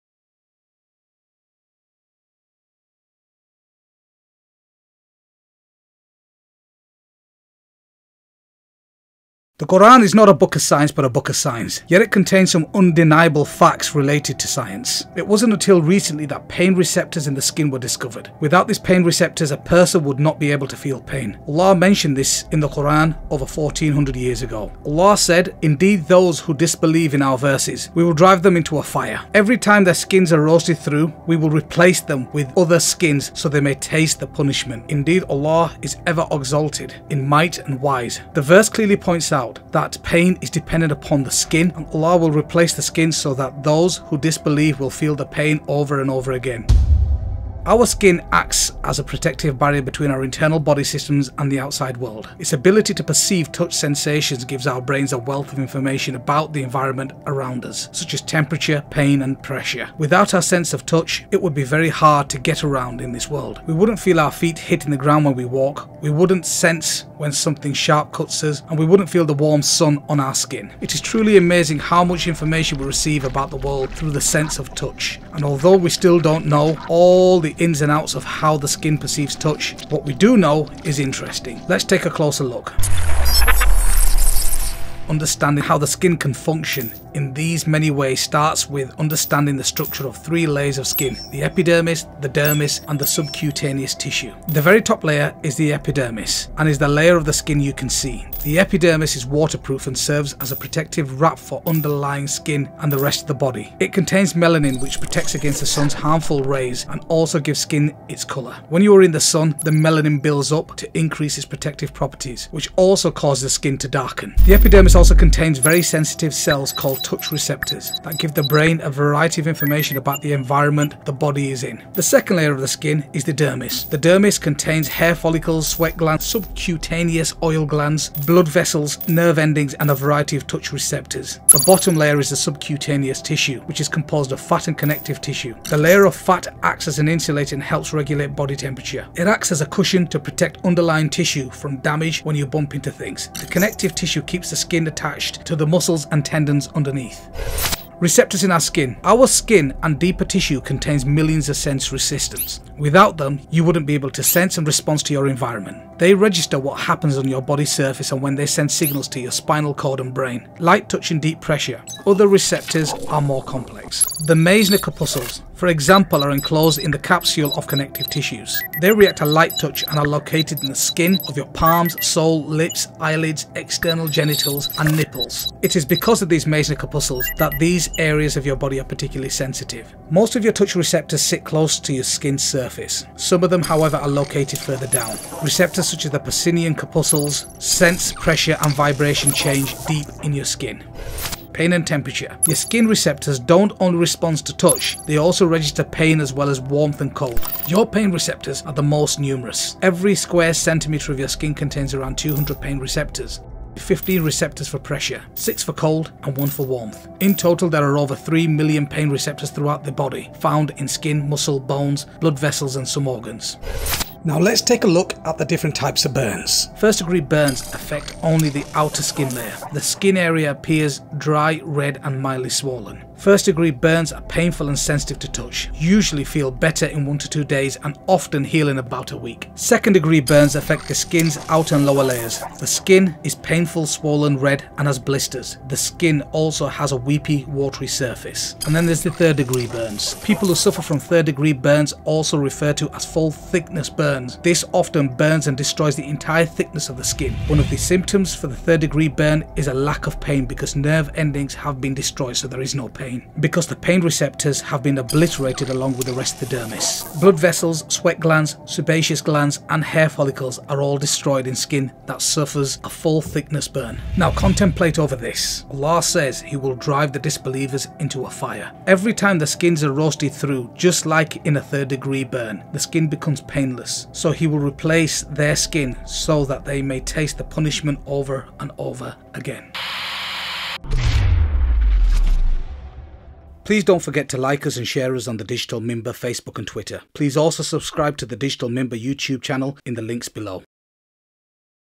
Pain Receptors - Shocking Facts
Personal Facebook MUST SEE! Scientific Facts in the Qur'an - Pain Receptors

The Qur'an is not a book of science but a book of signs, yet it contains some undeniable facts related to science.

It wasn't until recently